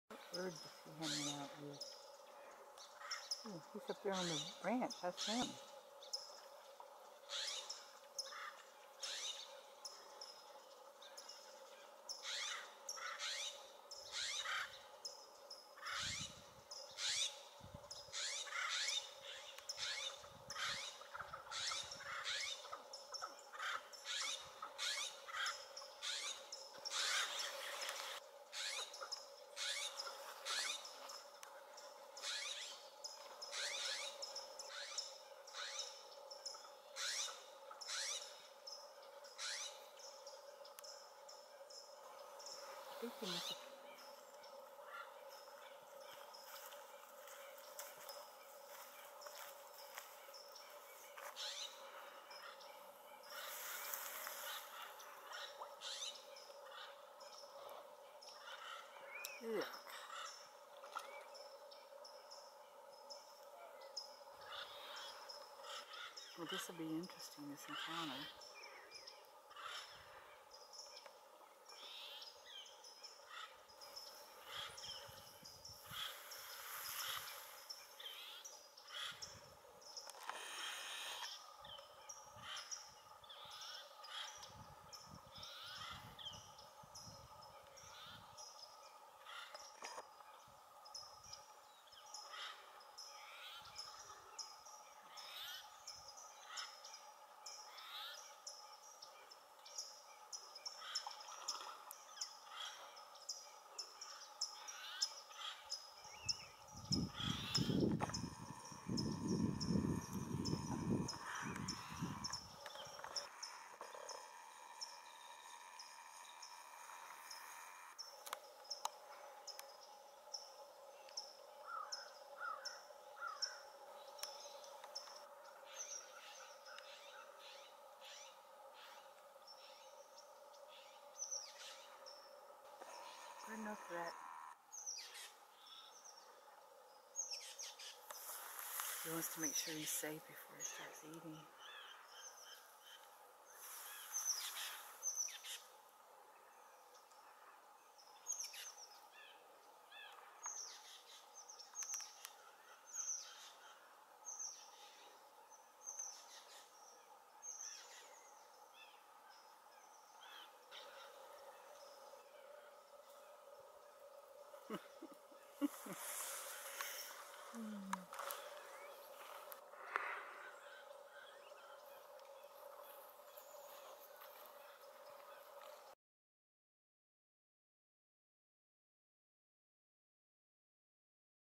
Otter in Kelsey Slough, December 7, 2021
The smell of the dead fish attracts other animals as the otter protect its feast.